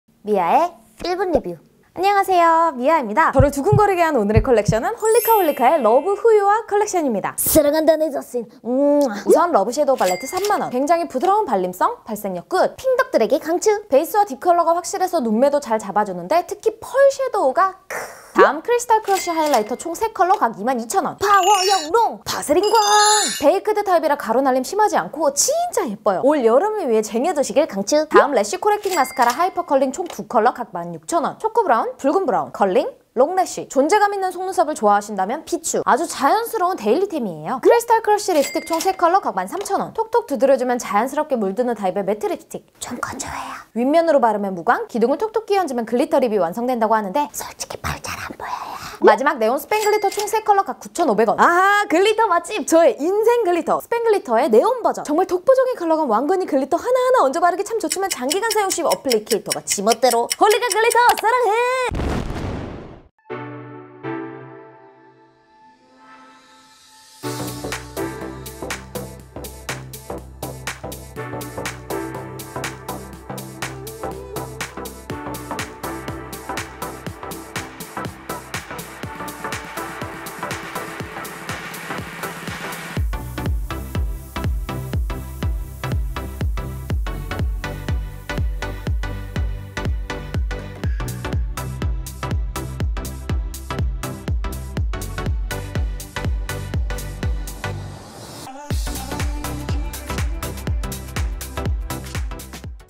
돌아온 미아의 1분리뷰! 홀리카홀리카 러브후유아 컬렉션😘
안녕하세요 미아입니다 다들 주말 잘 보내고 계신가요?
제가 홀리카홀리카 러브후유아 컬렉션 출시된거 보자마자
너무 마음에 들어서 바로 리뷰를 들고 오려고 했는데 조금 늦었네요😢
기다려주신 분들께 미안하구 고마워요ㅠㅠ!!!
이번 컬렉션은 의미도 좋지만 컬러감도 예뻐서 너무 귀엽더라구요.
제가 좋아하는 알록달록 무지개쓰...ㅋㅋㅋㅋ

간만에 돌아온 미아의 1분리뷰 재미있게 봐주셨으면 좋겠구
촬영 비하인드 스토리는 라이브인미아 채널에 업로드되어 있으니 놀러오세용 ㅋㅋ

그럼 저는 더 재미있고 더 새로운 영상으로 돌아올테니
우리 다음 영상에서 다시 만나요!
모두들 행복한 주말 보내세요❤️

제 인스타그램으로도 놀러오세요~